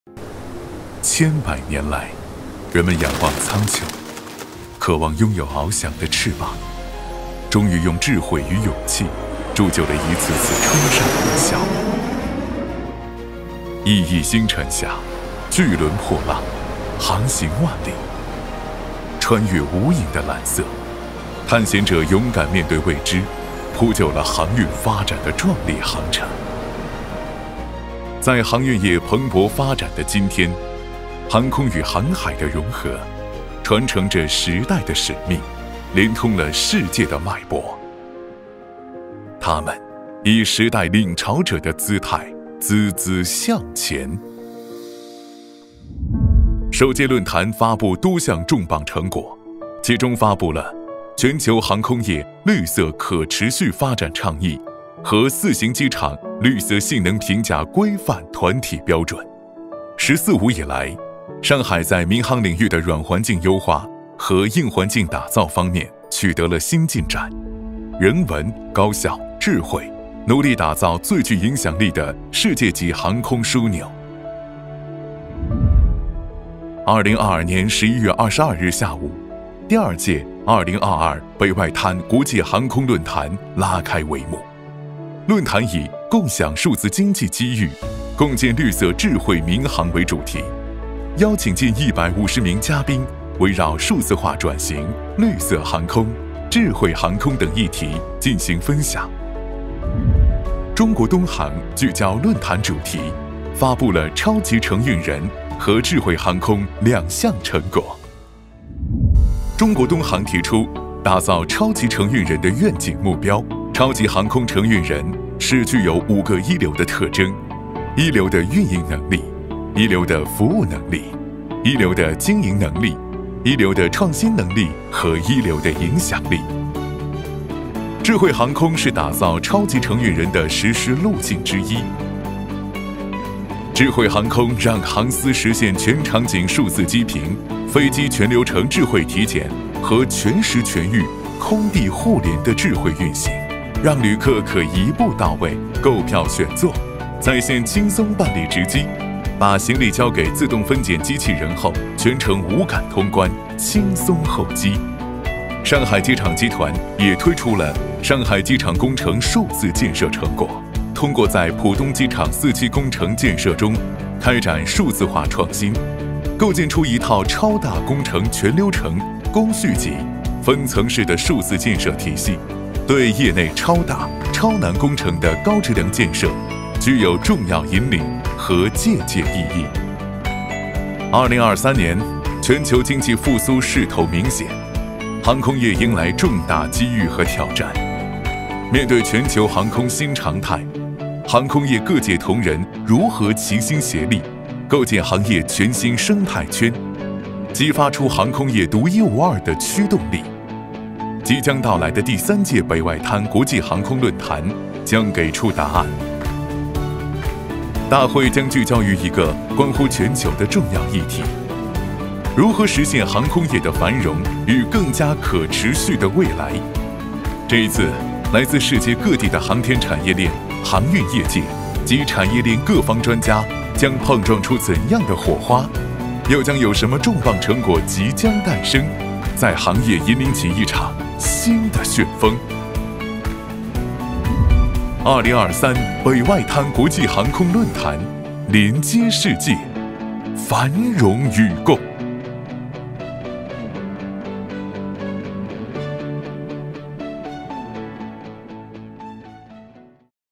Today, the 3rd North Bund International Aviation Forum will open. 🛫
Today, the 3rd North Bund International Aviation Forum will open. 🛫Experts from various sectors of this industry will gather to share insights about future aviation. 🌟A quick review of what we have achieved in the past forums.